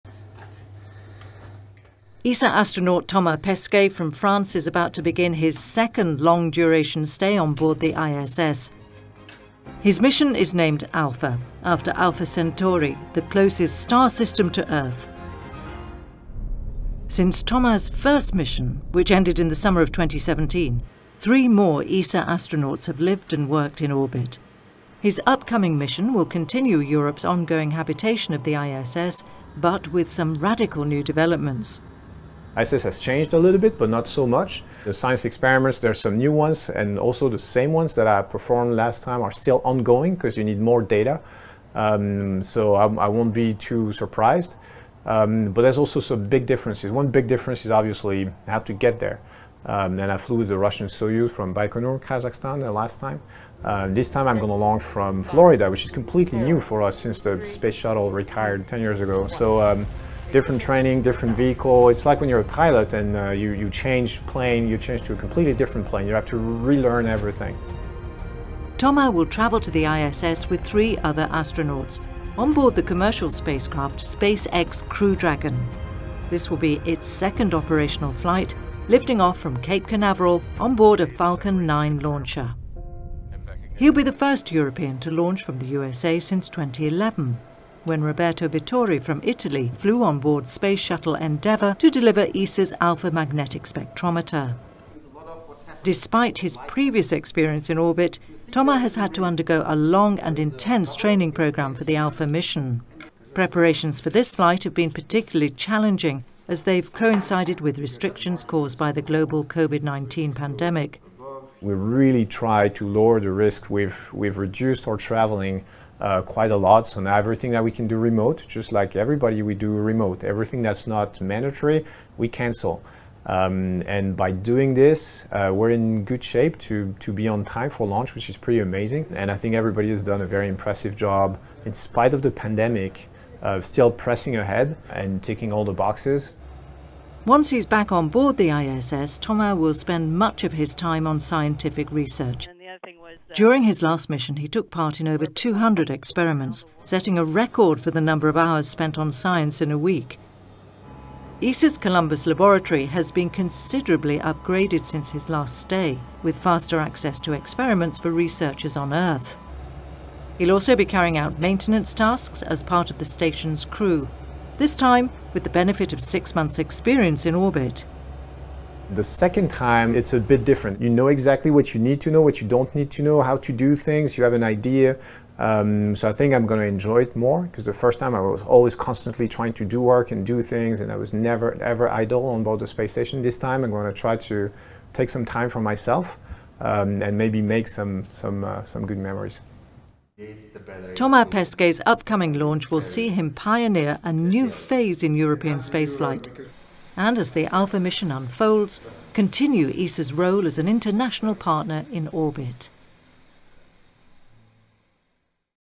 Préparation de la mission Alpha avec Thomas Pesquet
L'astronaute français de l'ESA Thomas Pesquet entamera bientôt sa deuxième mission vers l'ISS.

Nommée Alpha, la mission débutera par le deuxième vol opérationnel du SpaceX Crew Dragon, lancé depuis Cap Canaveral en Floride. Thomas sera le premier astronaute de l'ESA à voyager dans ce vaisseau spatial, qui restera amarré à la Station pendant environ six mois avant de renvoyer l'équipage sur Terre.